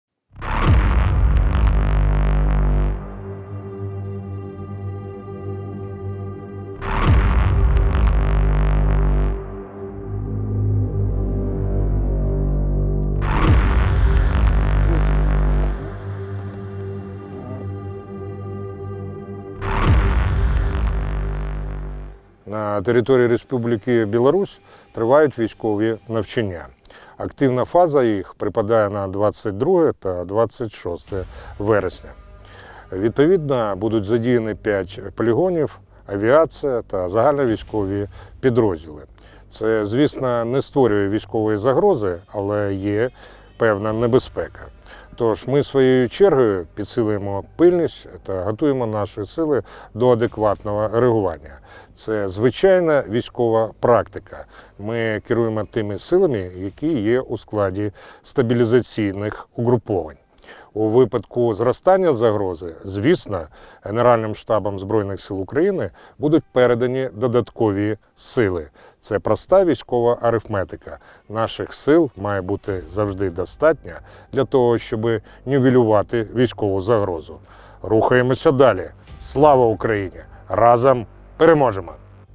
Наєв: білорусі тривають військові навчання, активна фаза яких припадає на 22 та 26 вересня
В навчаннях будуть залучені п'ять полігонів, авіація та загальновійськові підрозділи. Це не створює військової загрози, але є певна небезпека.

«Тож ми, своєю чергою, підсилюємо пильність та готуємо наші сили до адекватного реагування. Це звичайна військова практика», — зауважив Наєв.

Також він додає, що наших сил має бути достатньо для того, щоб нівелювати військову загрозу.